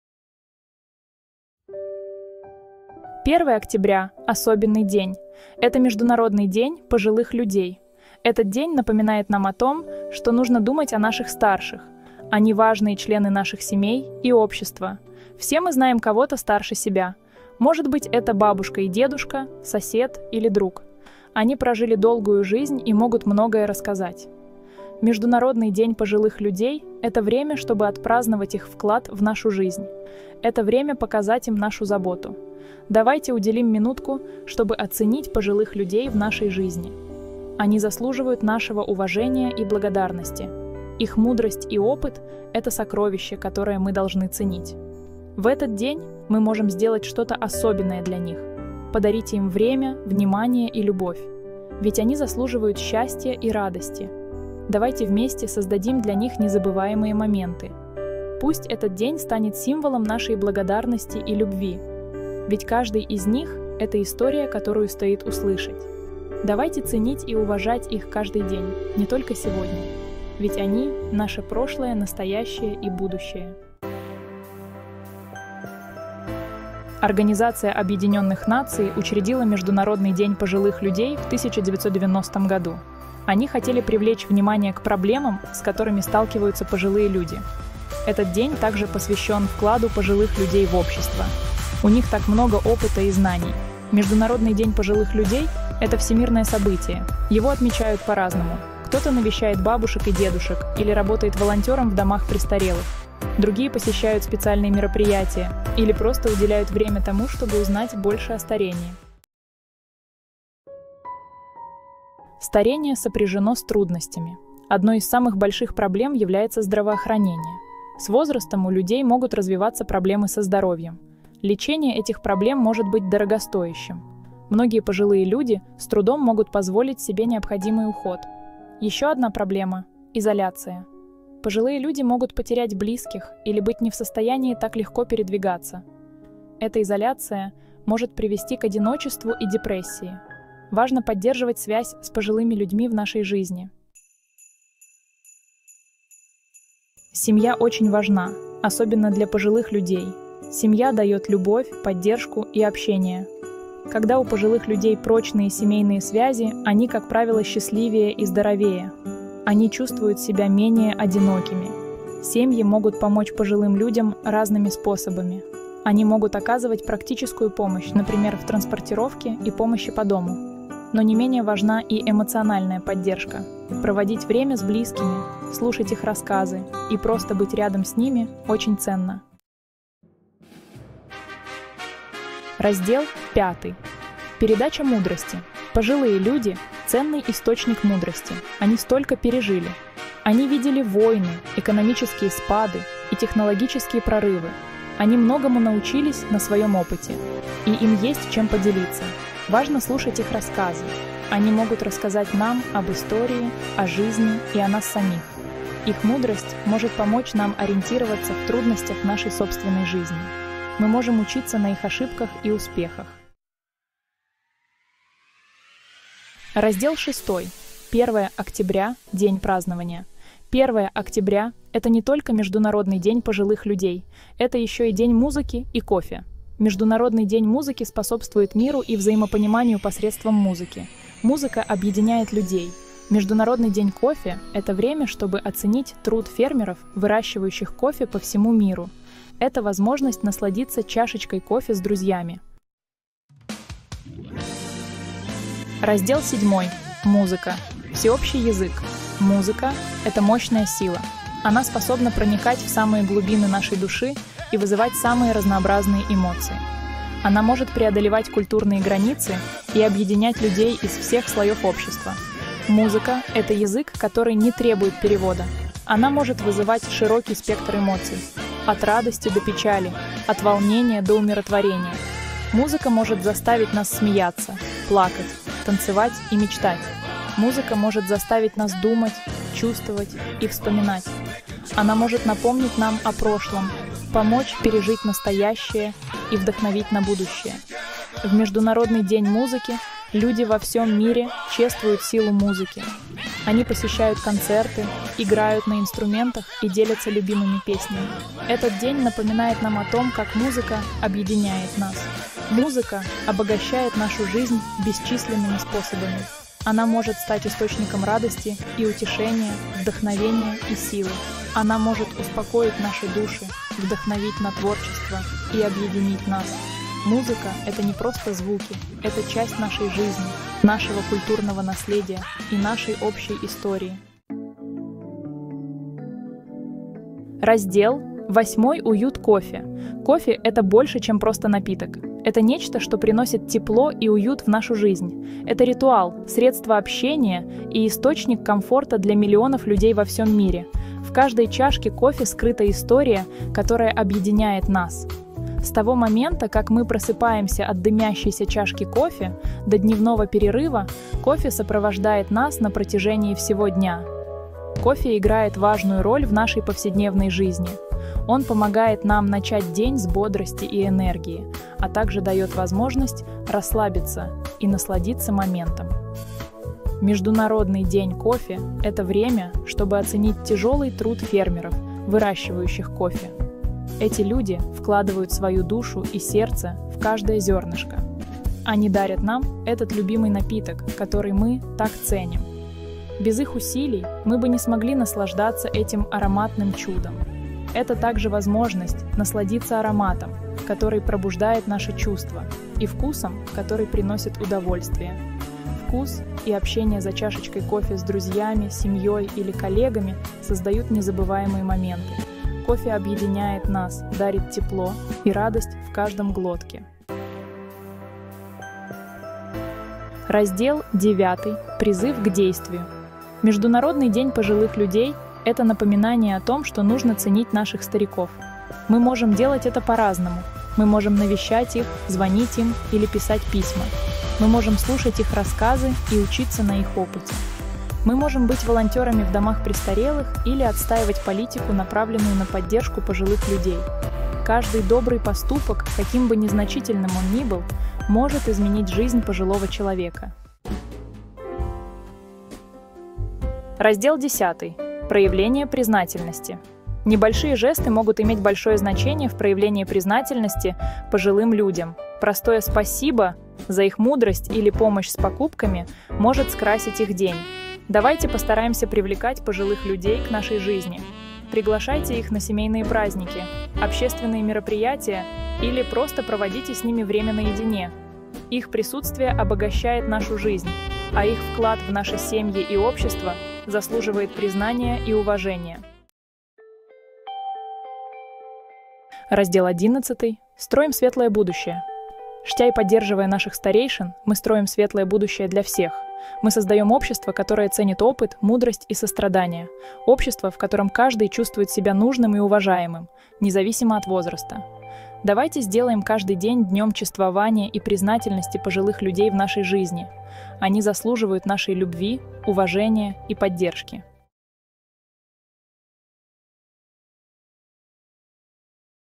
🎉 Святкуємо День пожилого человека 2024: Значення та традиції! 🎊
OUTLINE:
00:00:00 A Day for Everyone
00:01:13 Honoring Our Elders
00:01:49 The Challenges of Aging
00:02:23 The Importance of Family
00:02:59 Passing Down Wisdom
00:03:33 A Day of Celebration
00:04:02 A Universal Language
00:05:24 The Comfort of Coffee
00:06:47 A Call to Action
00:07:20 Showing Appreciation
00:07:56 Building a Brighter Future

People & Blogs

**Description:**
🌍 1 жовтня - це не лише День пожилого человека, а й свято, яке об'єднує людей по всьому світу! 🎈 Цього року ми святкуємо цю важливу дату, що нагадує про цінність старшого покоління та важливість сімейних зв'язків. 👵👴

💚 Уже з 1990 року цей день привертає увагу до проблем людей похилого віку, а також допомагає зберігати традиції, які передаються з покоління в покоління.

🎶 Серед інших свят, що відзначаються 1 жовтня, - Міжнародний день музики, Міжнародний день кави, Всесвітній день ходьби та Всесвітній день вегетаріанства! ☕🚶‍♂️🥗

🔔 Не пропустіть можливість долучитися до святкування та підтримати наших старших друзів і родичів! Організуйте зустріч, подаруйте невеличкий сюрприз або просто зателефонуйте, щоб висловити свою любов і повагу! ❤️